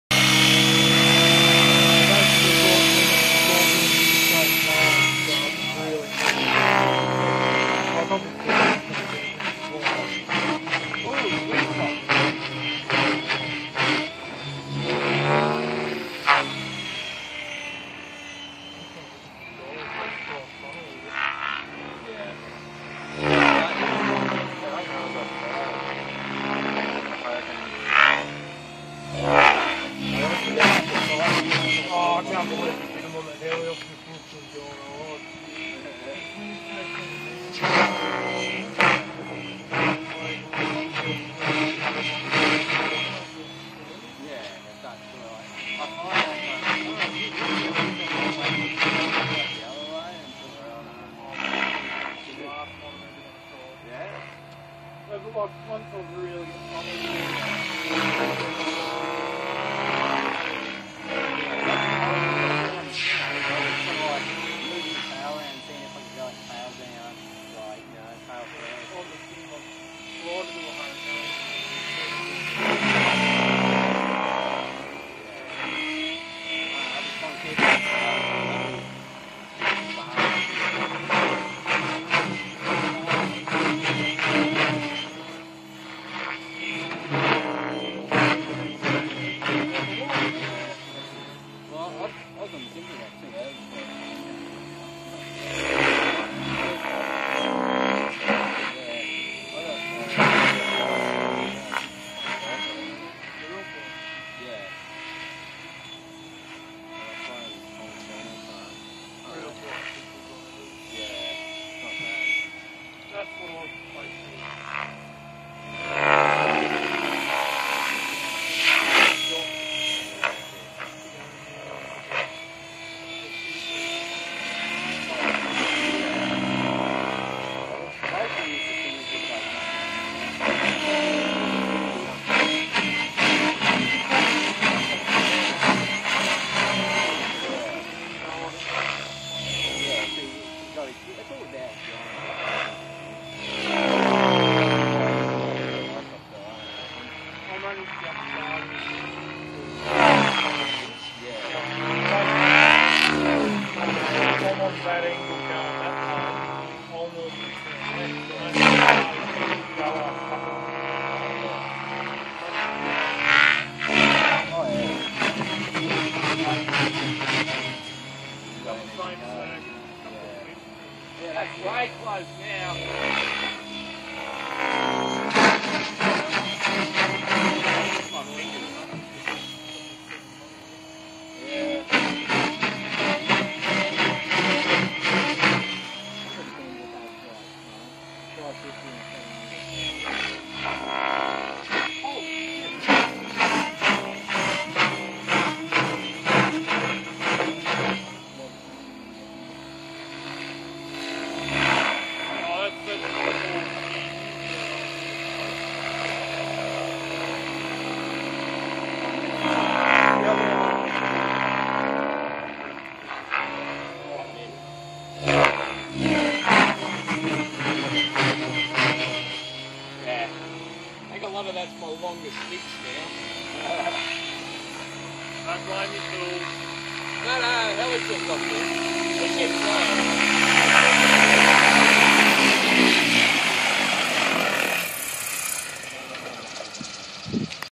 Gaui X7
Tom testing his new V-Bar setting on a Gaui X7 at Bunnings heli club.